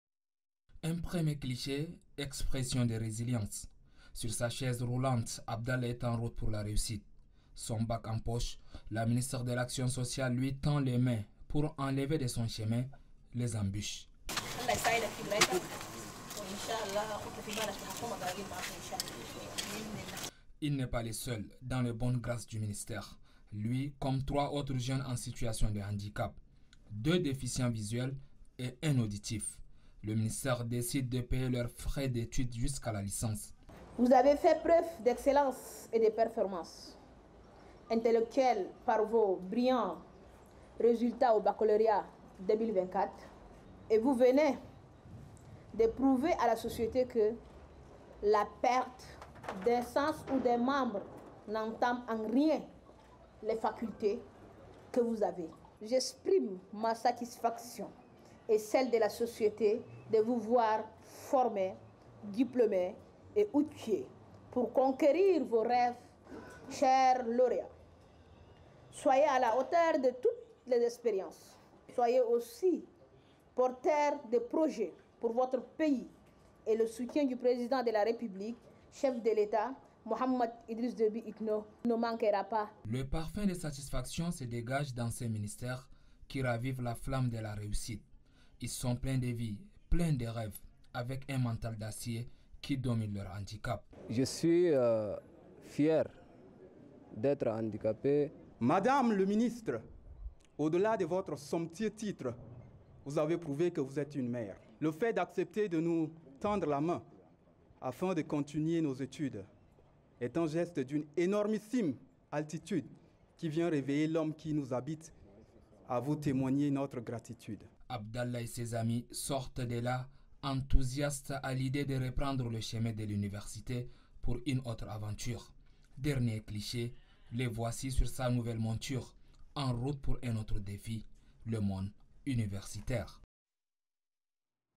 𝐄𝐍𝐒𝐄𝐈𝐆𝐍𝐄𝐌𝐄𝐍𝐓 𝐒𝐔𝐏𝐄𝐑𝐈𝐄𝐔𝐑
𝐋𝐞𝐬 𝐩𝐞𝐫𝐬𝐨𝐧𝐧𝐞𝐬 𝐞𝐧 𝐬𝐢𝐭𝐮𝐚𝐭𝐢𝐨𝐧 𝐝𝐞 𝐡𝐚𝐧𝐝𝐢𝐜𝐚𝐩, 𝐥𝐚𝐮𝐫𝐞́𝐚𝐭𝐬 𝐚𝐮 𝐛𝐚𝐜𝐜𝐚𝐥𝐚𝐮𝐫𝐞́𝐚𝐭 𝐩𝐚𝐬𝐬𝐞́ 𝐨𝐧𝐭 𝐫𝐞𝐜̧𝐮 𝐝𝐞𝐬 𝐛𝐨𝐮𝐫𝐬𝐞𝐬 𝐝'𝐞́𝐭𝐮𝐝𝐞𝐬 𝐣𝐮𝐬𝐪𝐮'𝐚̀ 𝐥𝐚 𝐥𝐢𝐜𝐞𝐧𝐜𝐞 𝐞𝐭 𝐝𝐞𝐬 𝐦𝐨𝐲𝐞𝐧𝐬 𝐦𝐚𝐭𝐞́𝐫𝐢𝐞𝐥𝐬 𝐞𝐧 𝐟𝐨𝐧𝐜𝐭𝐢𝐨𝐧 𝐝𝐞 𝐥𝐞𝐮𝐫 𝐡𝐚𝐧𝐝𝐢𝐜𝐚𝐩.  𝐂'𝐞𝐬𝐭 𝐮𝐧 𝐝𝐨𝐧 𝐝𝐮 𝐦𝐢𝐧𝐢𝐬𝐭𝐞̀𝐫𝐞 𝐝𝐞 𝐭𝐮𝐭𝐞𝐥𝐥𝐞 𝐪𝐮𝐢 𝐯𝐢𝐬𝐞 𝐚̀ 𝐥𝐞𝐮𝐫 𝐭𝐫𝐚𝐜𝐞𝐫 𝐥𝐞 𝐜𝐡𝐞𝐦𝐢𝐧 𝐝𝐞 𝐥𝐚 𝐫𝐞́𝐮𝐬𝐬𝐢𝐭𝐞. 𝐋𝐚 𝐜𝐞́𝐫𝐞́𝐦𝐨𝐧𝐢𝐞 𝐚 𝐞́𝐭𝐞́ 𝐩𝐫𝐞́𝐬𝐢𝐝𝐞́𝐞 𝐩𝐚𝐫 𝐥𝐚 𝐦𝐢𝐧𝐢𝐬𝐭𝐫𝐞 𝐝𝐞 𝐥'𝐚𝐜𝐭𝐢𝐨𝐧 𝐬𝐨𝐜𝐢𝐚𝐥𝐞 𝐅𝐚𝐭𝐢𝐦𝐞́ 𝐁𝐨𝐮𝐤𝐚𝐫 𝐊𝐨𝐬𝐬𝐞𝐢. 𝐀𝐥𝐲 𝐌𝐚𝐡𝐚𝐦𝐚𝐭 𝐁𝐞𝐥𝐥𝐨.